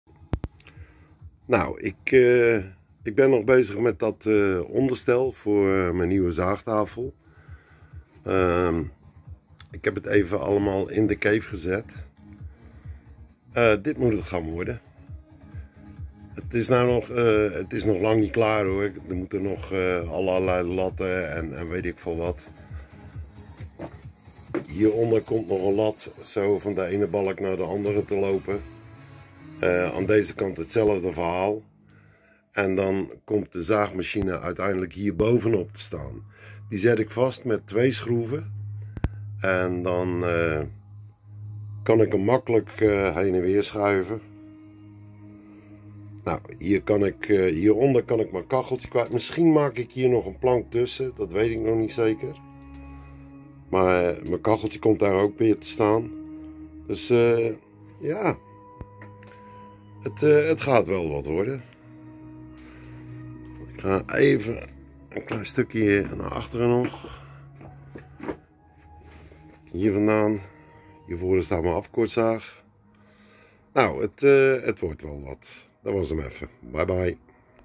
HBM 250 mm zaagtafel 4
Hallo allemaal.
Het nieuwe onderstel voor de zaagtafel begint al vorm te krijgen...ik denk dat het wel een mooi ding gaat worden.
Greets...Walther.